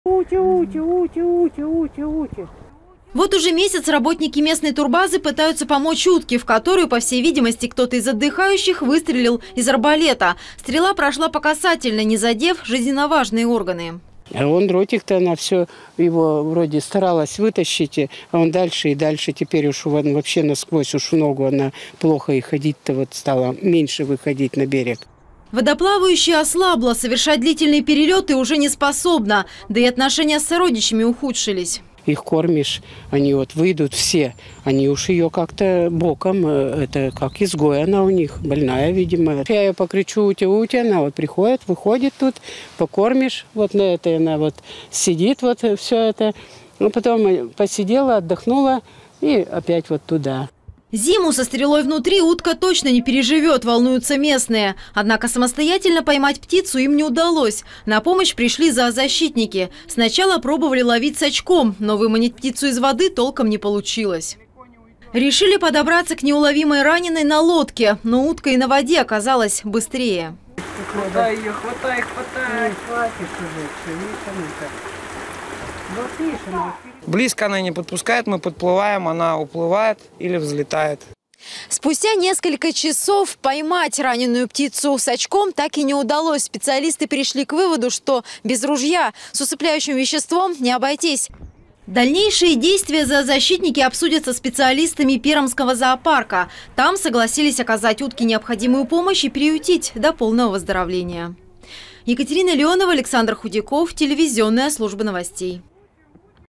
Пермяки спасают подстреленную из арбалета утку
Жители микрорайона Голый Мыс озаботились судьбой утки, подстреленной из арбалета. Водоплавающая долгое время живет буквально со стрелой в теле, улететь на зимовку она не в состоянии. Люди решили помочь раненной птице. За спасением утки наблюдала Екатерина Леонова.